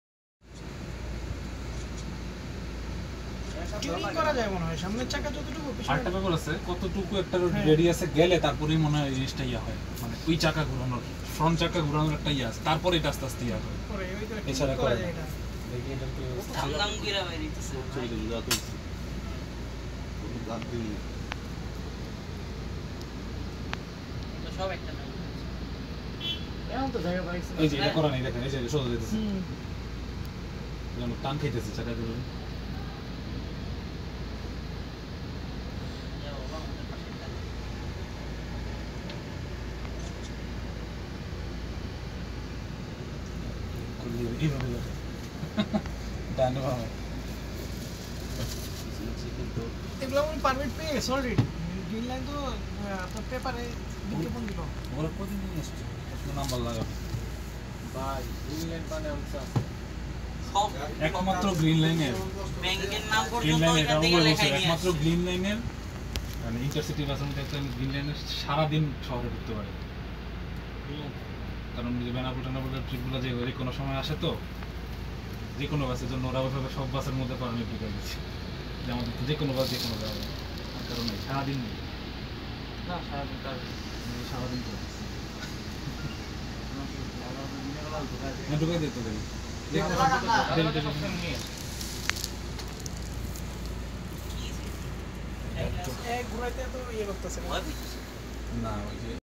GREEN LINE MAN DOUBLE DECKER @CHITTAGONG COUNTER
Green Line DD is leaving Chittagong counter for the first time .
Showdown Trip towards Dhaka